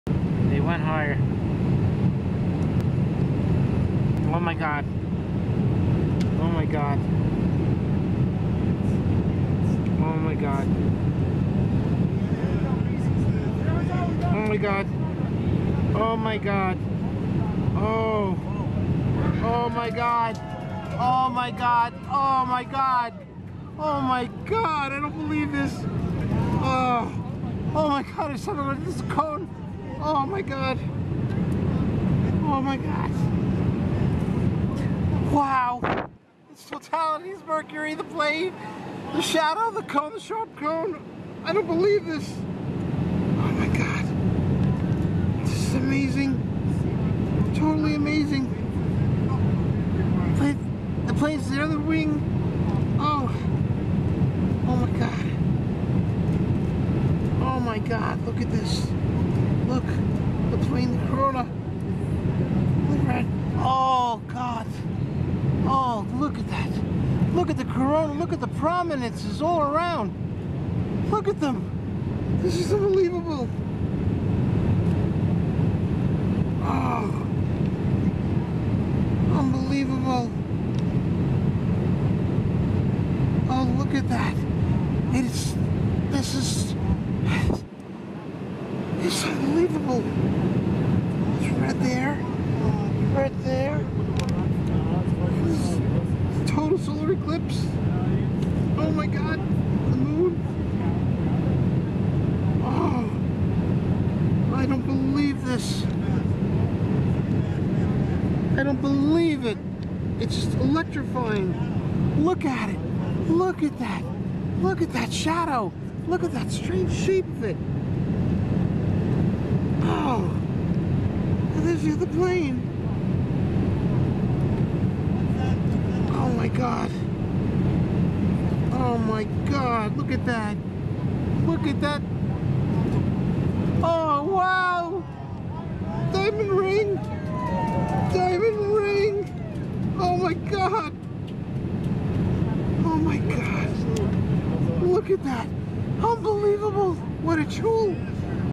Twin Dreamliners Intercept Eclipse Over Scotia Sea
On December 4, 2021 twin Dreamliners (B787-8) from LATAM Airlines, organized by TEI Global, rendezvoused with the shadow of the Moon. This was my second total solar eclipse from an airplane. I was so very lucky to go on this flight because of the generous financial support, suggestions, offerings, shares and encouragement of many friends. I first saw a scheduled airline flight that intercepted the shadow of the Moon on March 9, 2016. That video ended up being seen around the world. At the last minute I was able to board one of these Dreamliners. Not knowing what to expect, I once again got really excited unexpectedly, but in a different way, and I may have surpassed the amount of OMGs than the one from five years ago. It was chaotic but became pure bliss and touched me deeply. There was no better choreographed air and celestial sky show all out of one window frame!